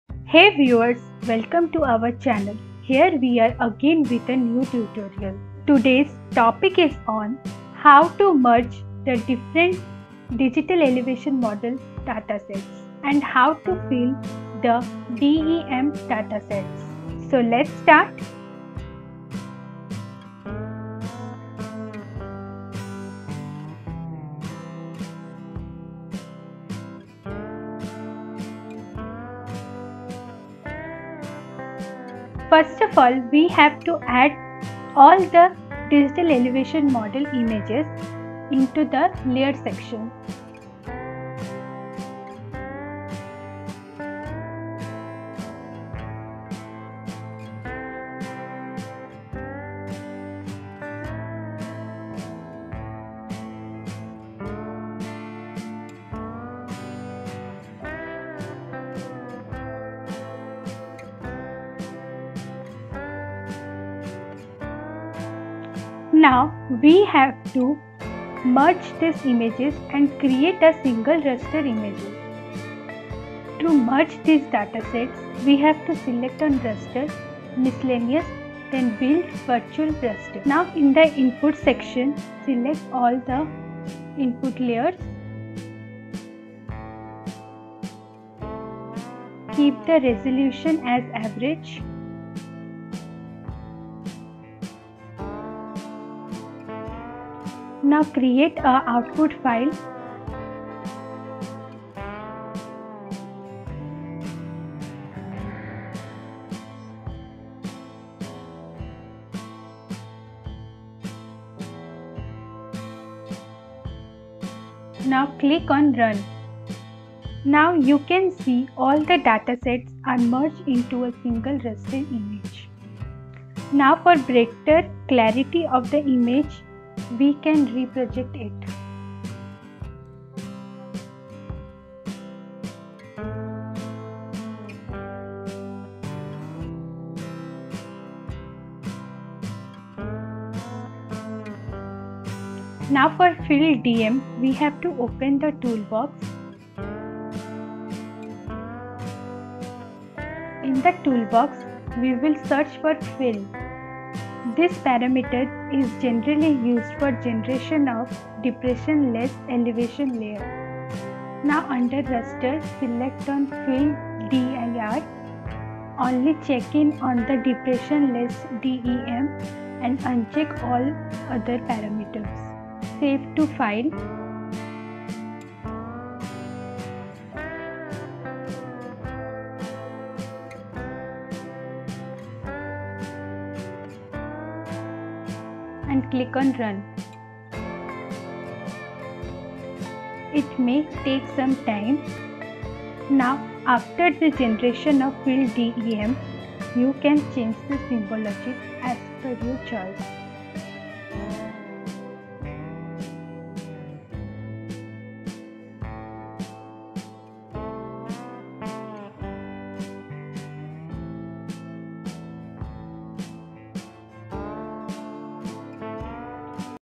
How to Mosaic DEM images & Fill the voids in DEM | qgis Basics Class 11
Welcome to QGIS Basics Class 10! In this tutorial, we will learn how to mosaic Digital Elevation Model (DEM) images and fill the voids to create a seamless and complete representation of the terrain. Whether you're a GIS enthusiast, a geospatial professional, or a student, this video will equip you with the essential skills to work with DEM data in QGIS.

In the first part of the tutorial, we will explore the process of mosaicking DEM images. Mosaicking involves combining multiple DEM files to create a larger, continuous DEM. We will demonstrate step-by-step instructions on how to import and load the DEM images into QGIS, align and reproject them if necessary, and use the mosaic tool to create a unified DEM.

Next, we will address the issue of voids or gaps that often occur in DEM data due to various reasons such as sensor limitations or data acquisition gaps. These voids can adversely affect the accuracy and visual representation of the terrain. We will show you how to identify and locate the voids in the DEM using QGIS tools.

To fill the voids, we will utilize several techniques available in QGIS. We will guide you through the process of using interpolation methods to estimate and fill the missing elevation values within the void areas. You will learn how to choose the appropriate interpolation method based on the characteristics of your data and the desired level of accuracy.

By the end of this tutorial, you will have a comprehensive understanding of how to mosaic DEM images and effectively fill the voids in DEM data using QGIS. These skills are essential for various applications, including landscape analysis, hydrological modeling, and site suitability assessments.

Let's dive into the world of QGIS and enhance our geospatial skills together!

Chapters
00:00 Intro
00:23 Mosaic DEM Images
02:03 Reproject Raster
02:22 Fill Voids in DEM
03:21 Outro